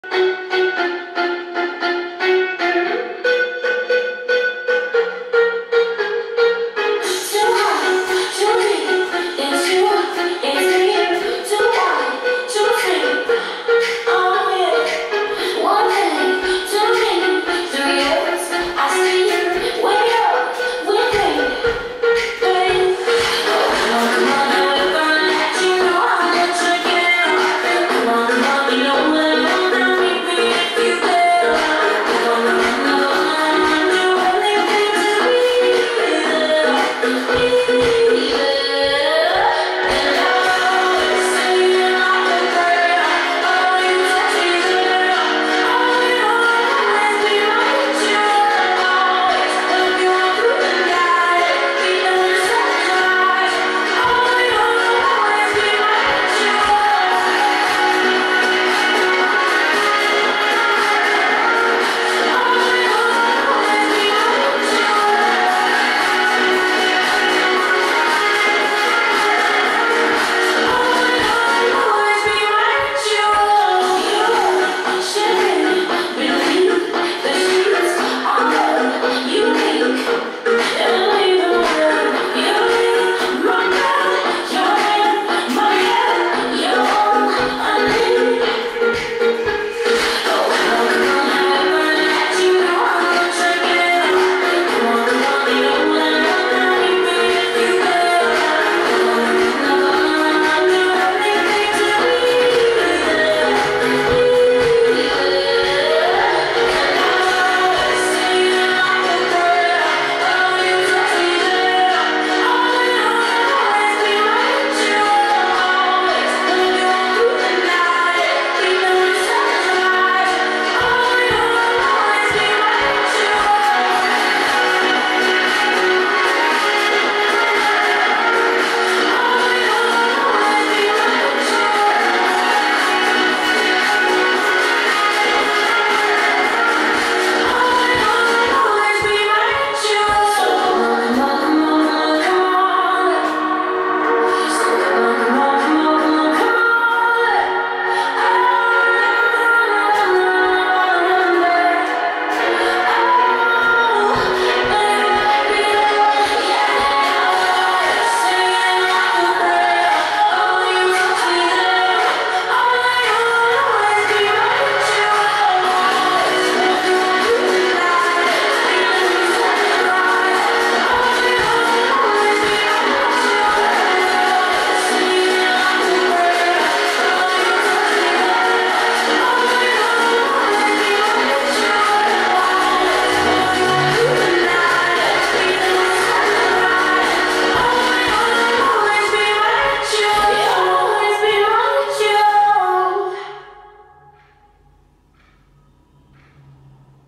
My Ritual - Line Dance
Choreographed by: Niels Poulsen (Dec 2019)
32 count - 4 wall - funky Beginner level line dance